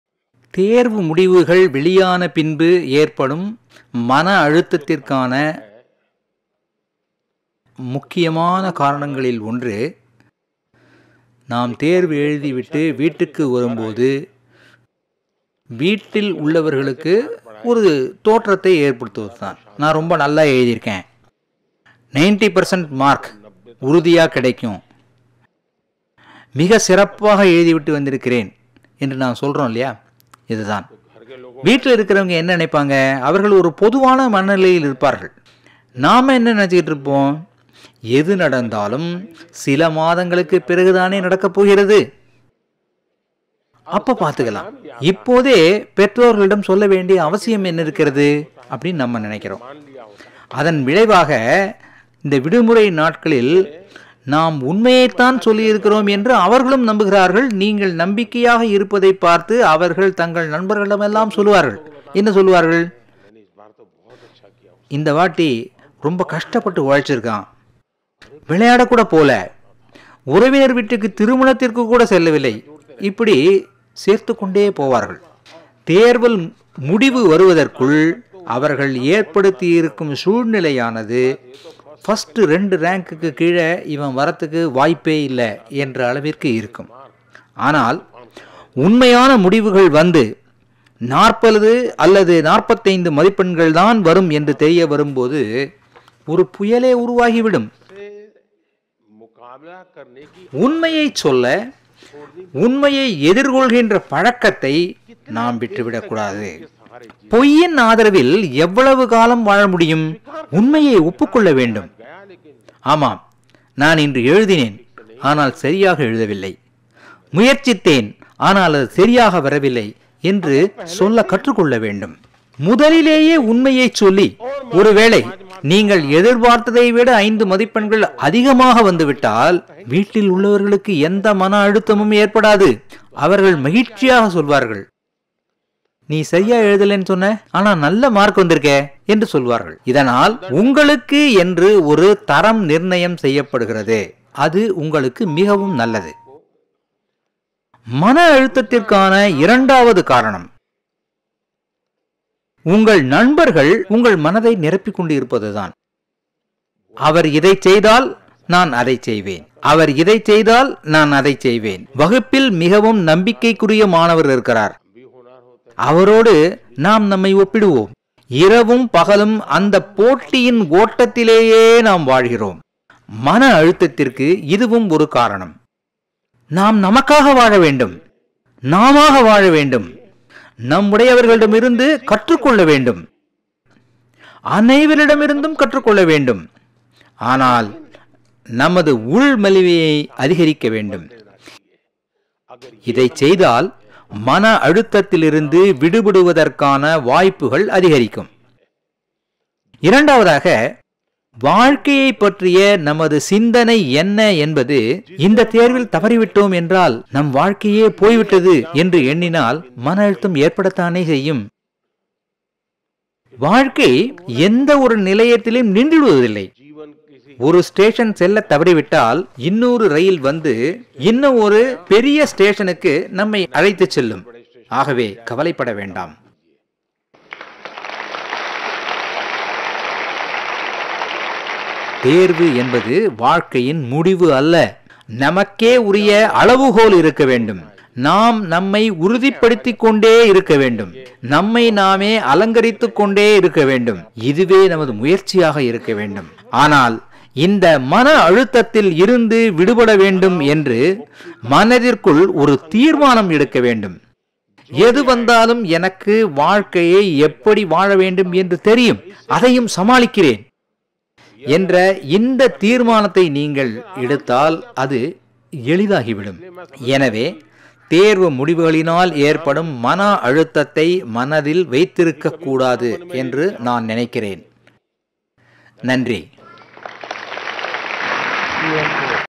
தேர்வு முடிவுகள் வாழ்க்கையின் முடிவு அல்ல - பிரதமர் மோடி
Narendra Modi PM Modi
Exam results are not the end of life, says PM Modi-Tamil

Pariksha Pe Charcha 2023 Interactions in Tamil